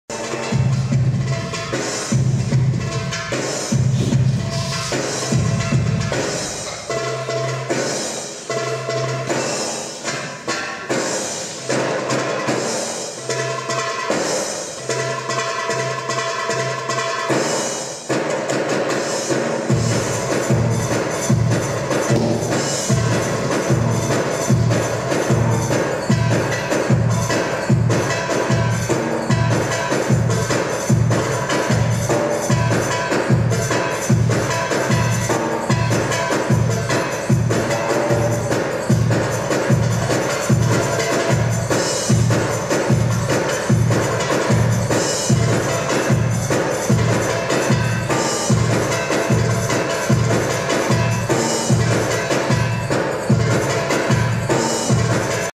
Патока Екатерина
Екатерина Патока "Зажигательная табла. Секреты уникальной техники Мастера, разнообразные методы и приемы выразительных хореографических средств, в том числе - о работе диафрагмы. Постановка"